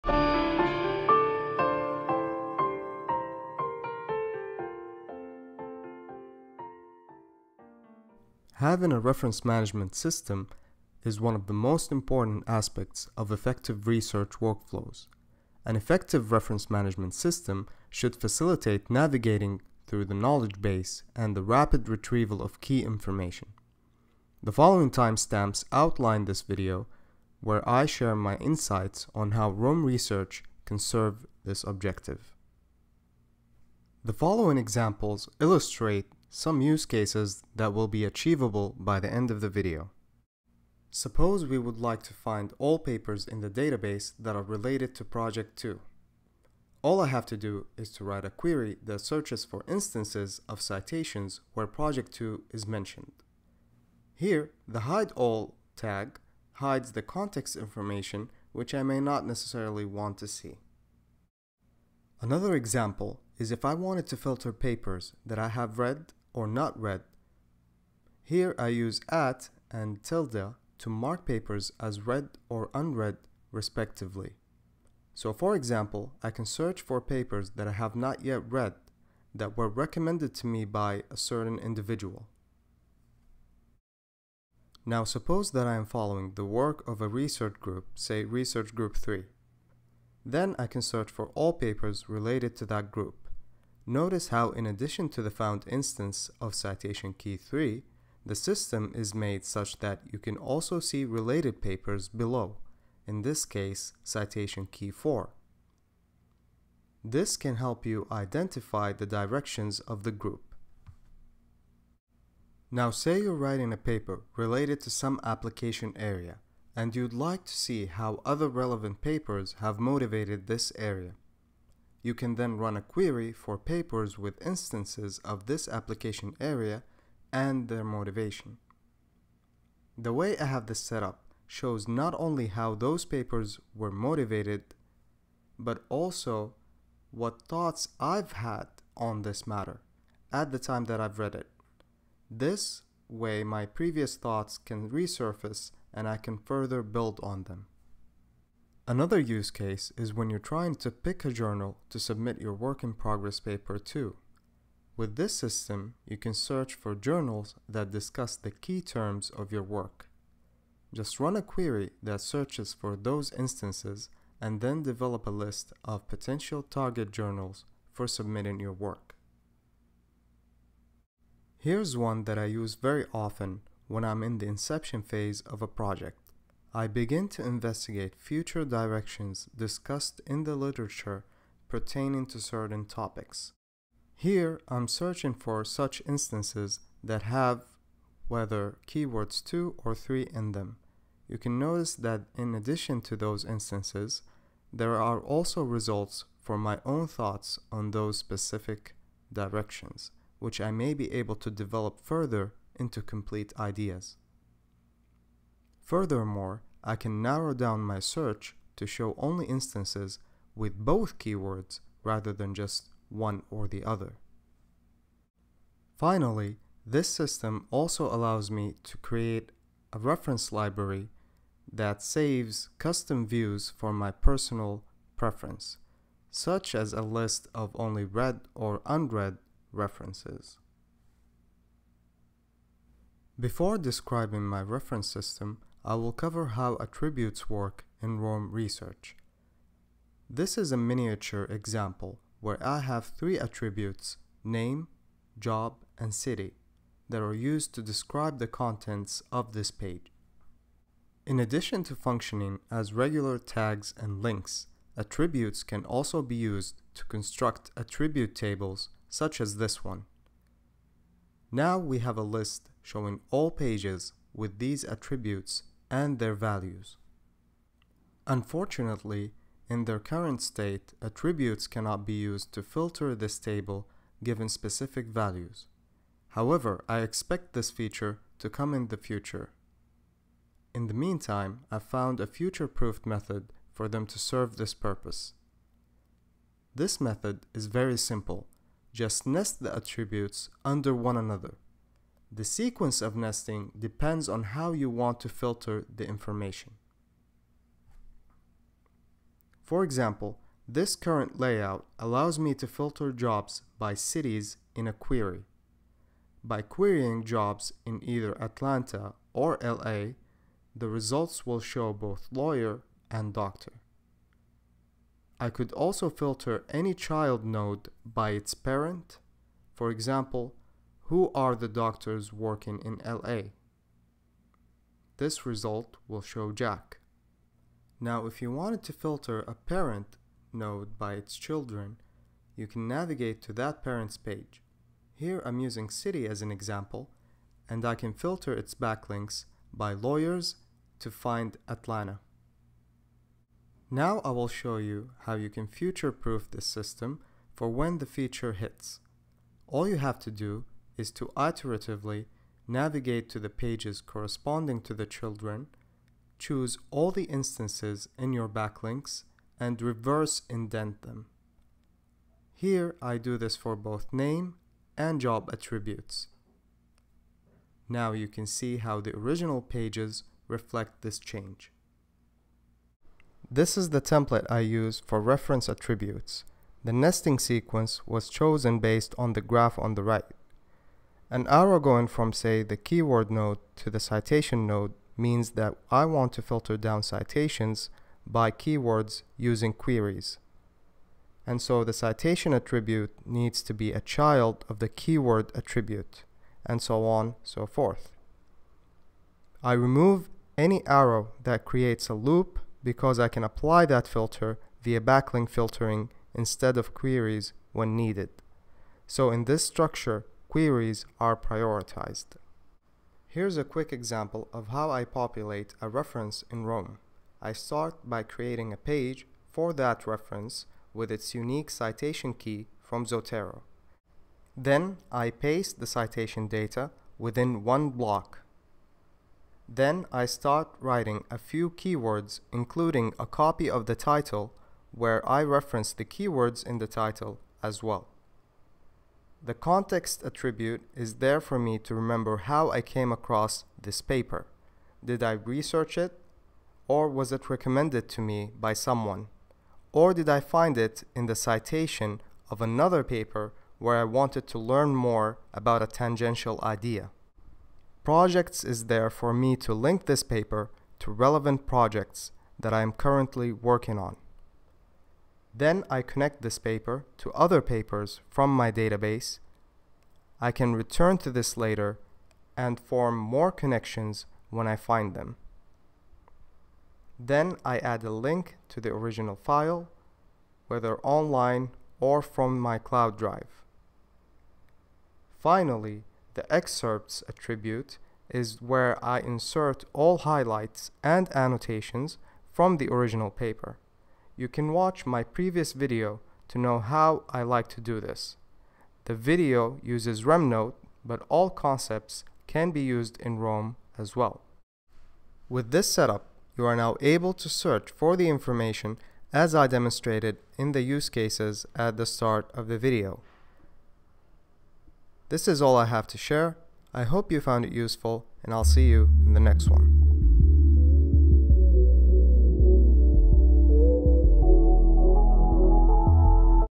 Future Proof Attributes for Roam Research: Academic Workflow Demo
An essential part of academic research workflow is a reference management system. Therefore, developing an effective system that is capable of frictionless retrieval of key information is essential for a productive workflow. In this video, I illustrate how Roam Research can be used for this purpose.
RemNote - Roam Zotero Citation Meta Data Style:
Video outline:
System Use case Demonstration     0:40
How Attributes Work in Roam          3:54
Reference Management System      6:08